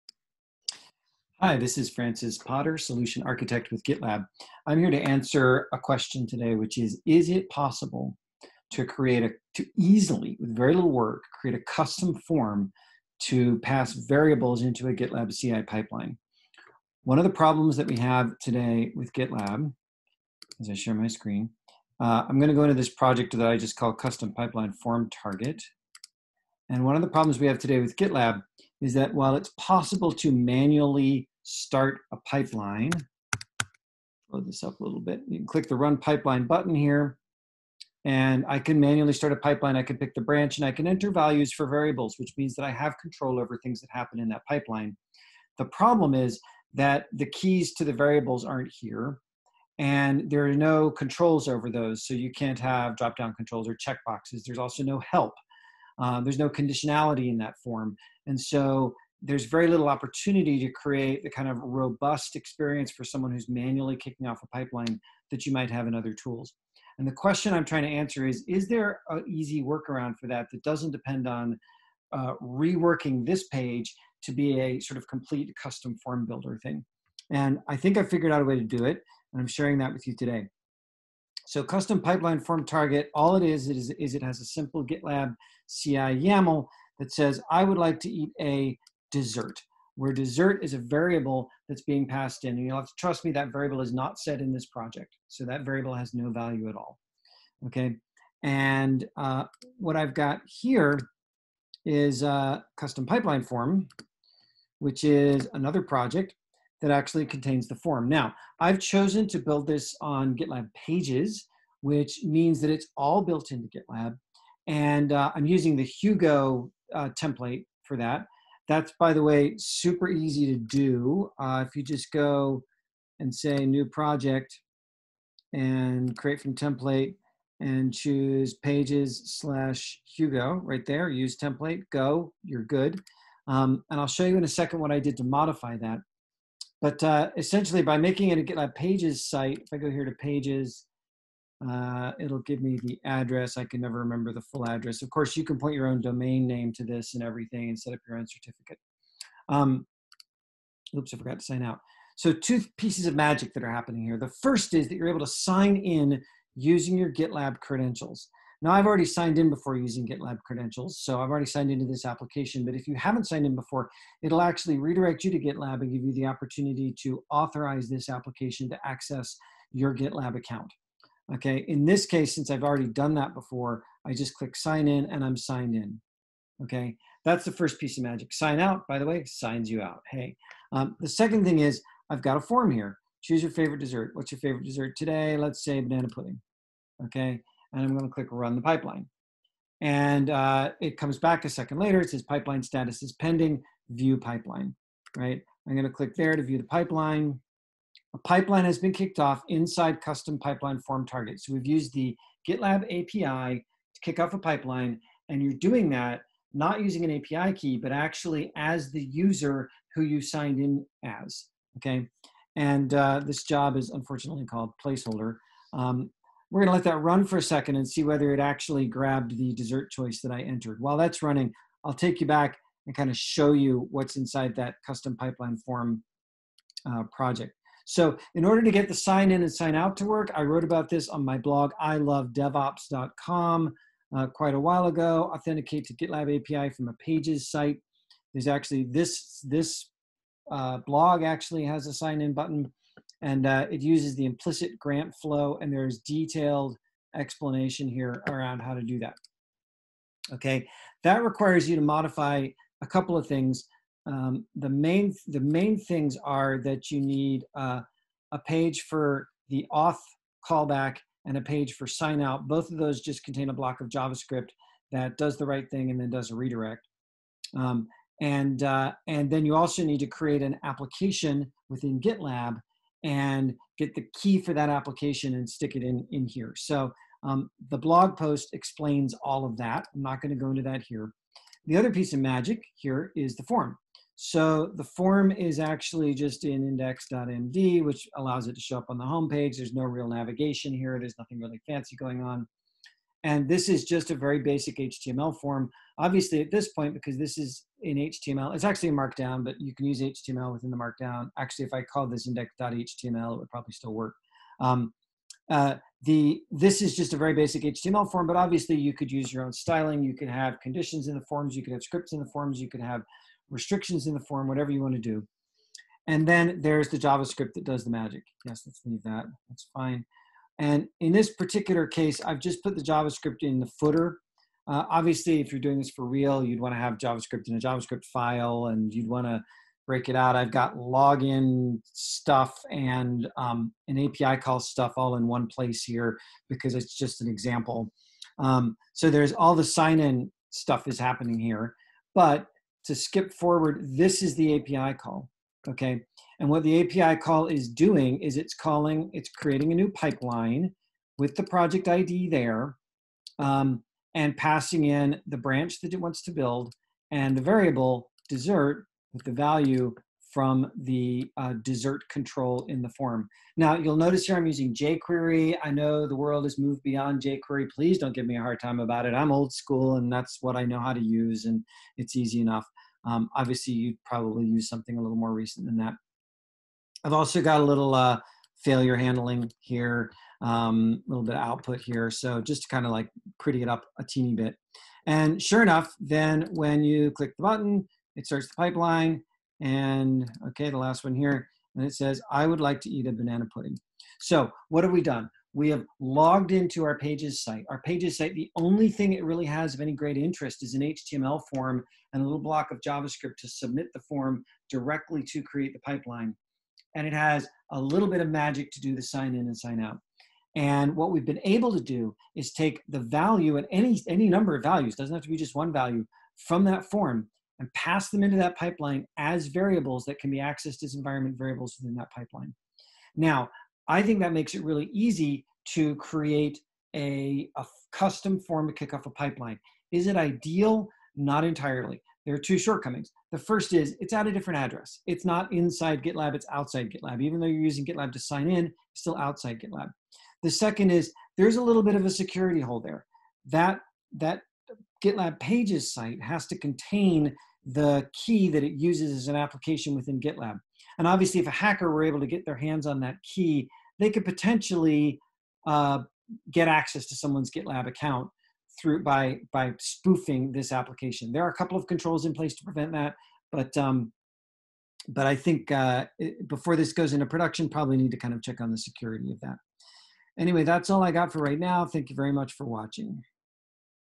Custom Pipeline Form for GitLab CI/CD inside a GitLab Pages site
Recorded by Francis Potter

Since the video was recorded, some of the need for the functionality demonstrated has gone away as GitLab CI/CD itself got stronger. For example, GitLab now support variable descriptions and options to make the manual pipeline run form more usable. And GitLab Pages itself now supports access control. Plus, the technique used in the video -- Implicit Grant Flow -- opens security holes. Now GitLab supports PKCE, which is designed to act similarly but more securely.